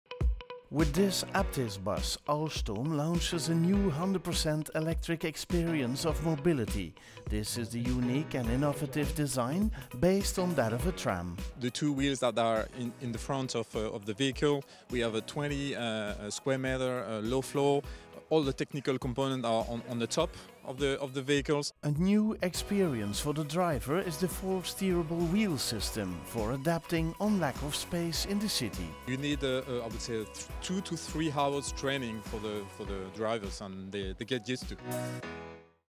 Busworld Europe 2017 - Aptis Alstom NTL
With this Aptis bus Alstom launches a new 100% electric experience of mobility.